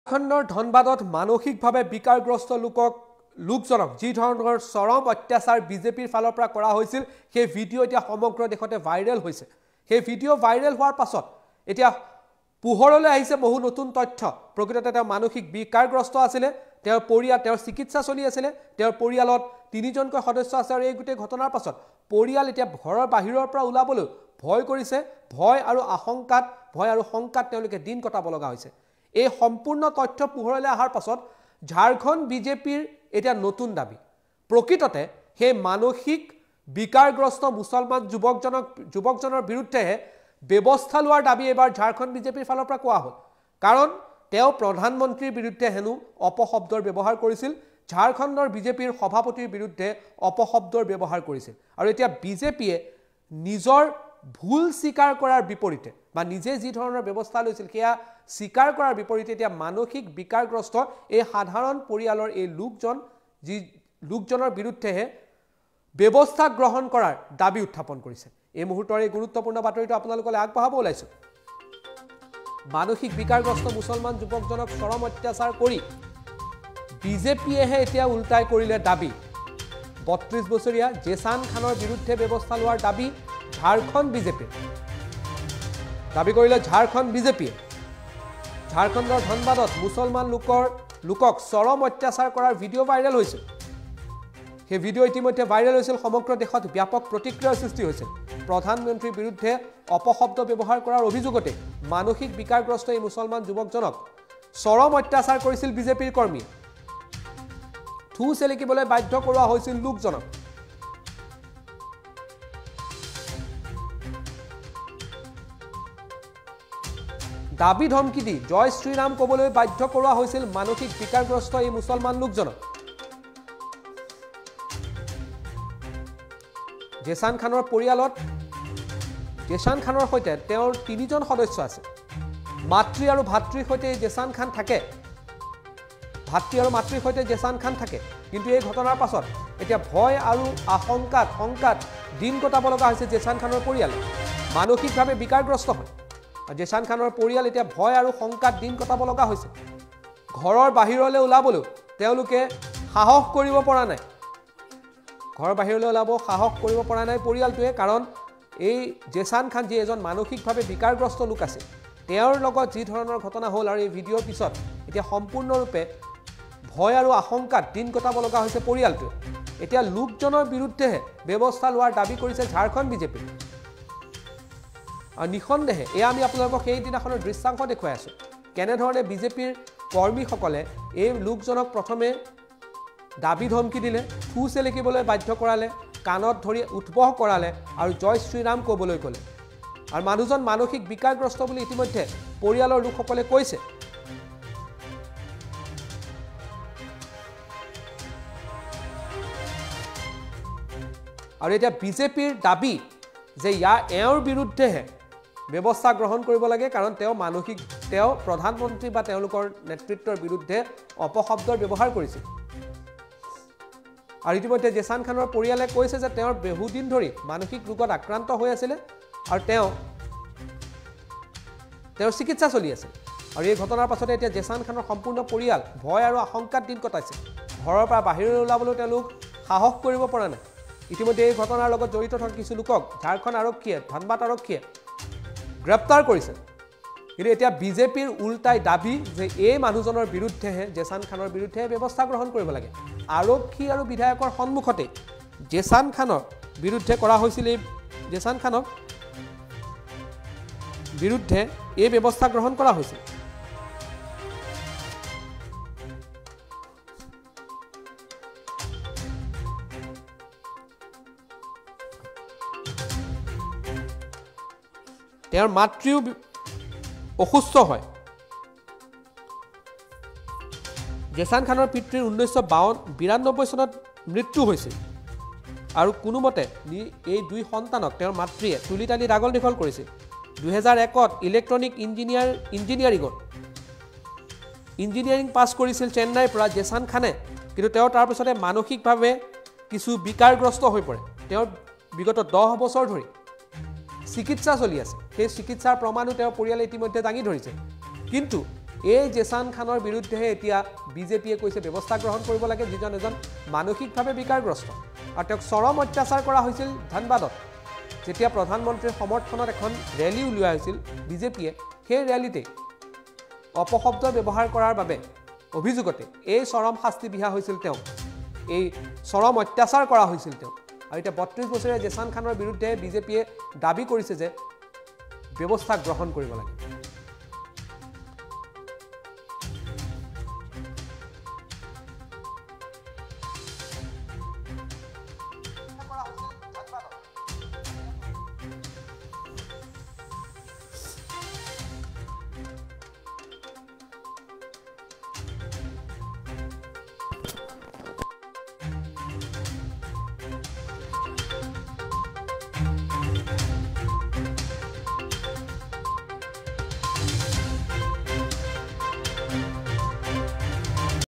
মানসিক বিকাৰগ্ৰস্ত মুছলমান যুৱকক চৰম অত্যাচাৰ কৰি বিজেপিয়ে কৰিলে ওলোটাই দাবী।
মানসিক বিকাৰগ্ৰস্ত মুছলমান যুৱকক চৰম অত্যাচাৰ কৰি বিজেপিয়ে কৰিলে ওলোটাই দাবী। ৩২ বছৰীয়া জেচান খানৰ বিৰুদ্ধে ব্যৱস্থা লোৱাৰ দাবী বিজেপিৰ।